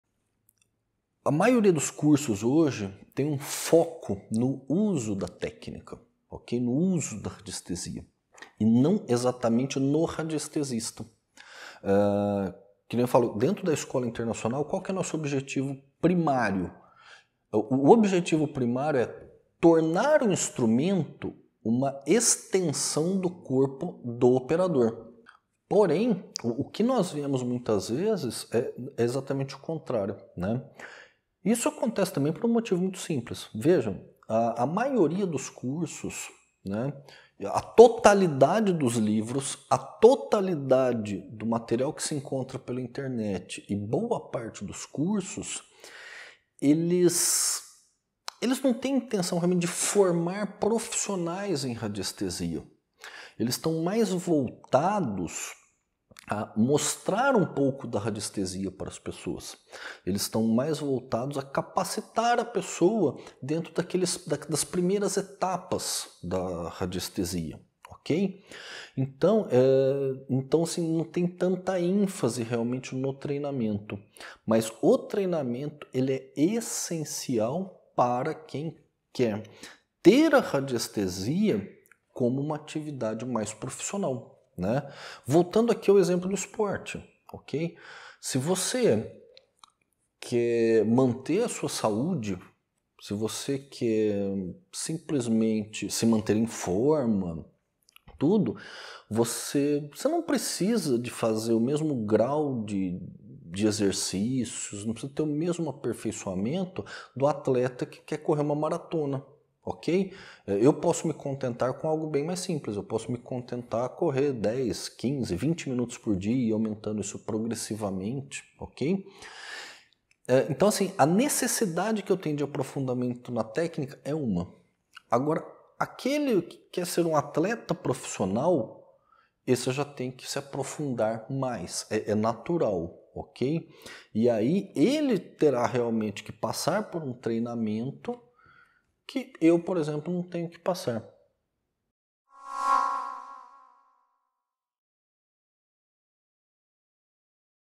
Qual é o foco de nossos cursos de Radiestesia?
Neste vídeo explico de forma sucinta o foco dos cursos que divulgamos na Escola Internacional de Radiestesia, e mostro o quão importante é que o operador desenvolva determinadas bases em suas práticas, que facilitarão seu progresso e domínio das técnicas.